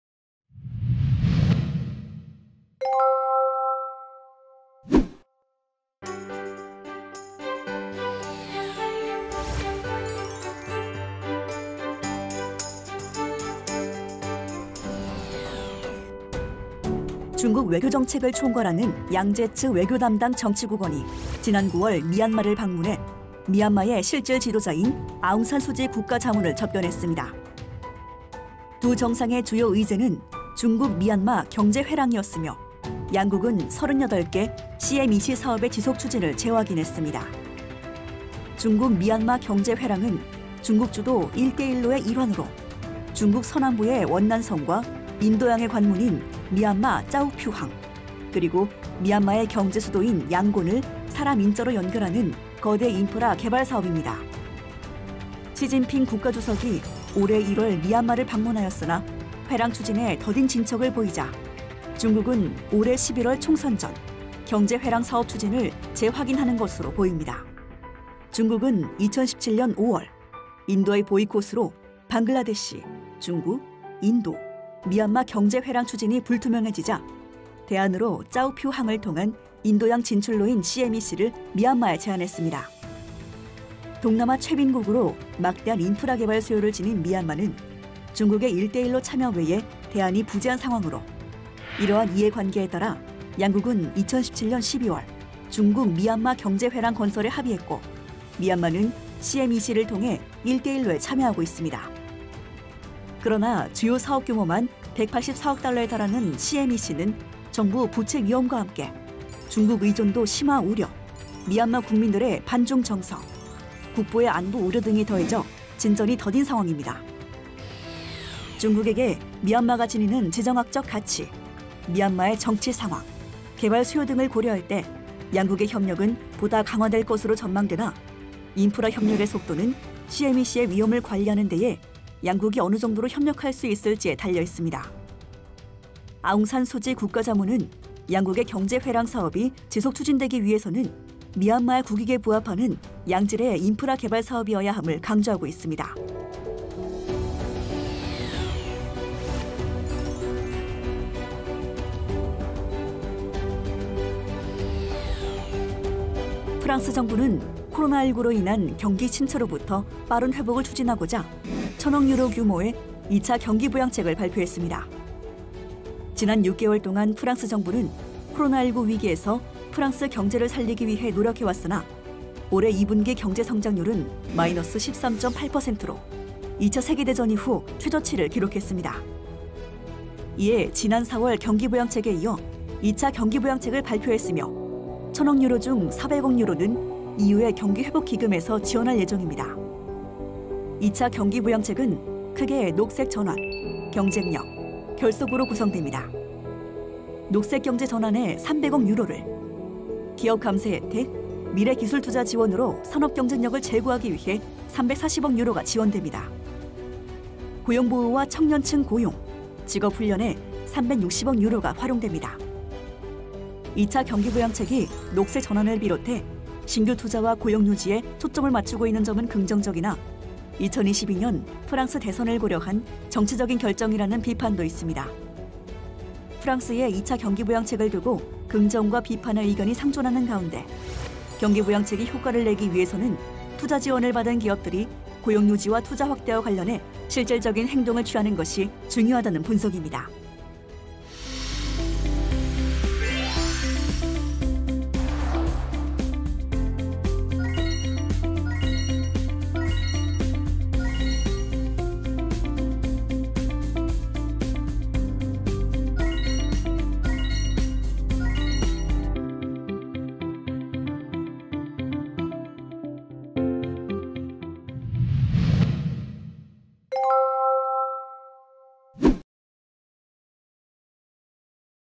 [Vol.35]중국-미얀마 경제회랑, 양국 모두에 이익 될까?/ 프랑스, 1,000억 유로 더 푼다... 2차 경기부양책 발표로 코로나19 회복 총력
🌍"KIEP 글로벌 이슈브리핑 코너"로
금주 살펴볼 곳은 "중국-미얀마"와 "프랑스" 입니다.😃

✔️중국-미얀마 경제회랑, 양국 모두에 이익 될까?(00:07~02:26)
✔️프랑스, 1,000억 유로 더 푼다... 2차 경기부양책 발표로 코로나19 회복 총력(02:27~04:00)
✔️그 외 해외이슈(04:02~04:20)

☑️중국-미얀마/ 중국-미얀마 경제회랑, 양국 모두에 이익 될까?(00:07~02:26)

중국 외교정책을 총괄하는 양제츠 외교담당 정치국원이 지난 9월 미얀마를 방문해,
미얀마의 실질 지도자인
아웅산 수지 국가자문을 접견했습니다.

두 정상의 주요 의제는
'중국-미얀마 경제회랑(CMEC)'이었으며,
양국은 38개 CMEC 사업의
지속 추진을 재확인했습니다.

중국에게 미얀마가 지니는 지정학적 가치,
미얀마의 정치상황, 개발 수요 등을 고려할 때,
양국의 협력은 보다 강화될 것으로 전망되나,
인프라 협력의 속도는
CMEC의 위험 관리에 있어,
양국이 어느 정도로 협력하느냐에 달려있습니다.

☑️프랑스/ 프랑스, 1,000억 유로 더 푼다...
2차 경기부양책 발표로 코로나19 회복 총력(02:27~04:00)

프랑스 정부는 코로나19로 인한
경기침체로부터 빠른 회복을 추진하고자,
1,000억 유로 규모의
2차 경기부양책(France Relance)를 발표했습니다.

녹색경제 전환에 300억 유로를,
기업 감세혜택, 미래 기술 투자 지원으로 산업 경쟁력을 제고하기 위해 340억 유로가 지원됩니다.
고용보호와 청년층 고용, 직업훈련에
360억 유로가 활용됩니다.

2차 경기부양책이 녹색전환을 비롯해
신규투자와 고용유지에 초점을 맞추고 있는 점은 긍정적이나, 2022년 프랑스 대선을 고려한
정치적인 결정이라는 비판도 있습니다.